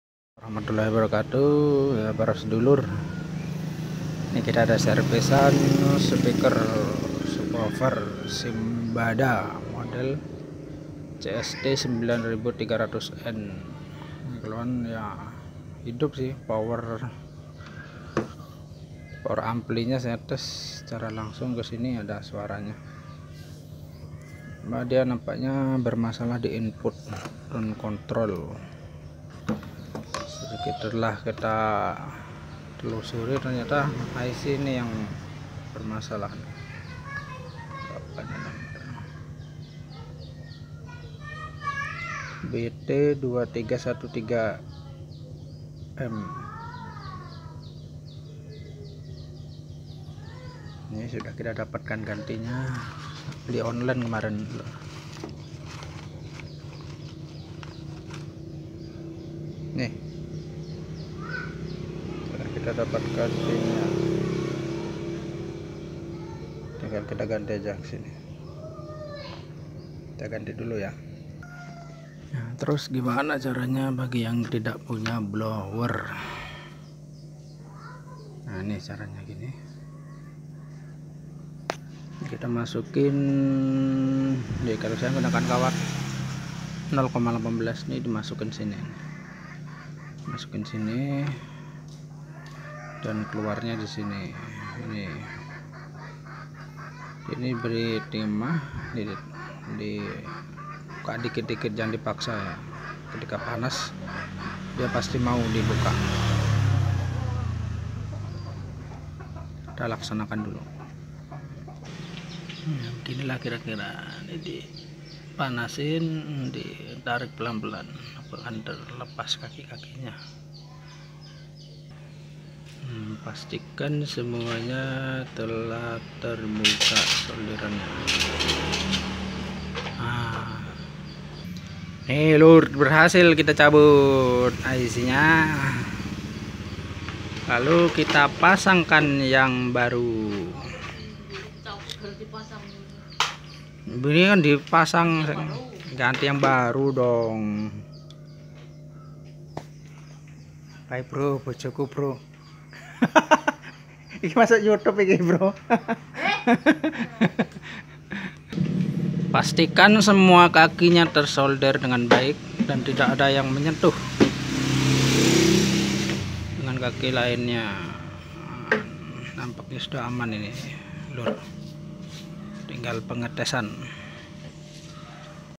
CARA SERVICE Speaker Aktif Simbadda CST 9300N Tidak ada suara Part 1
speaker ini kondisinya hidup tapi tdk bisa di input suara, krn ditest bagiam amplifiernya normal. ternyata ic Tone controlnya yg rusak dan ic inputnya juga rusak.